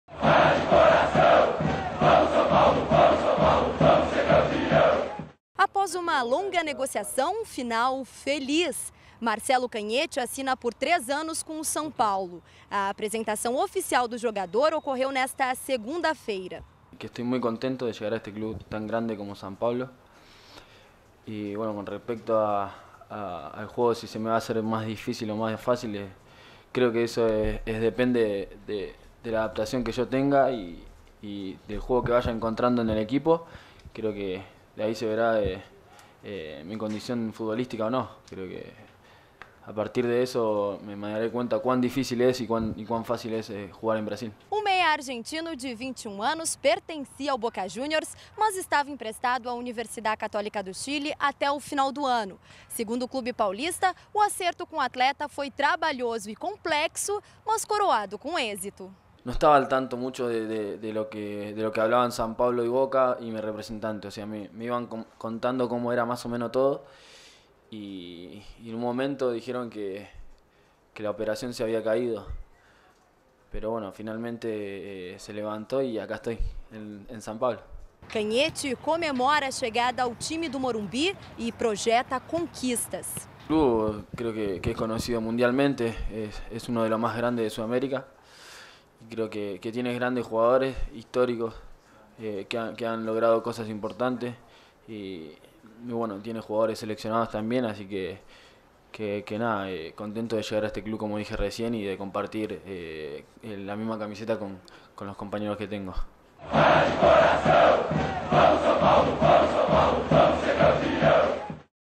SPFC - Tricolor apresenta Cañete
Meia argentino chega ao São Paulo e afirma que está muito feliz em vestir a camisa tricolor